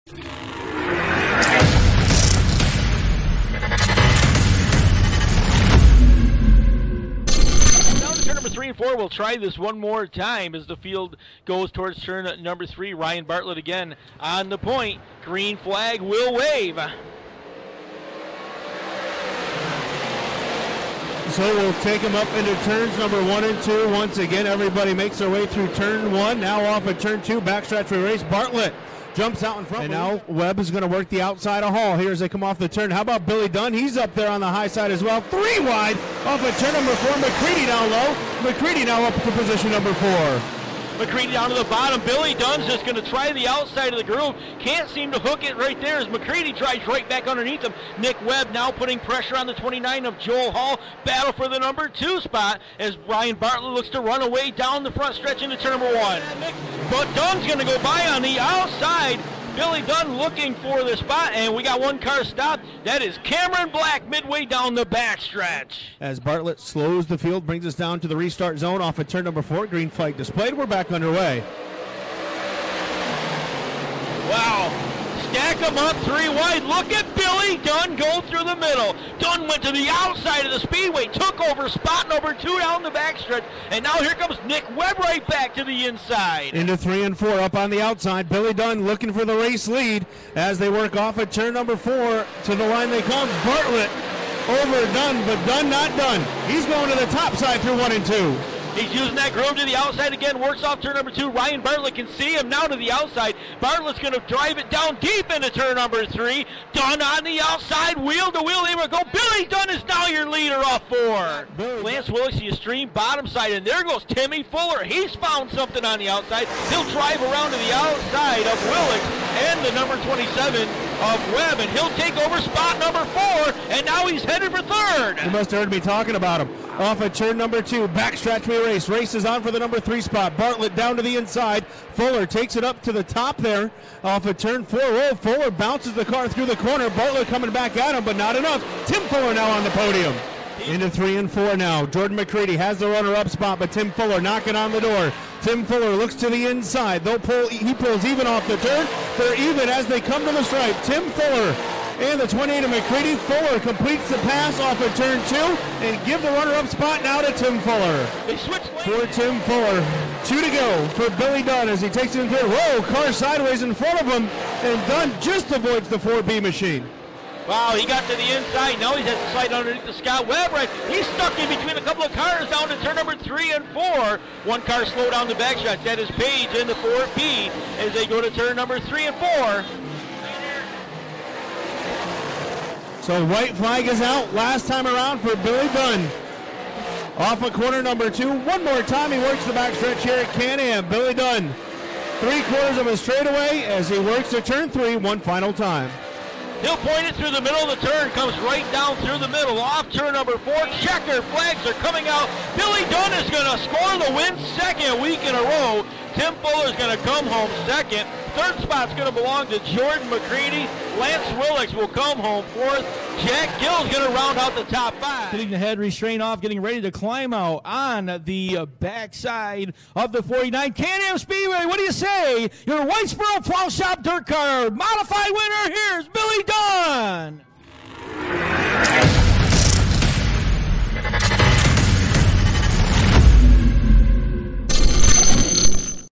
Can-Am Speedway | DIRTcar 358-Modified Feature Highlights | 5/28/21
Highlights from Can-Am Speedway on Friday, May 28, 2021.

Billy Dunn went back-to-back on Friday night dominating the 25-lap DIRTcar 358-Modified feature in picking up the win over Tim Fuller.

Watch racing events from around the Northeast live on Dirt Track Digest TV including weekly action at the Can-Am Speedway.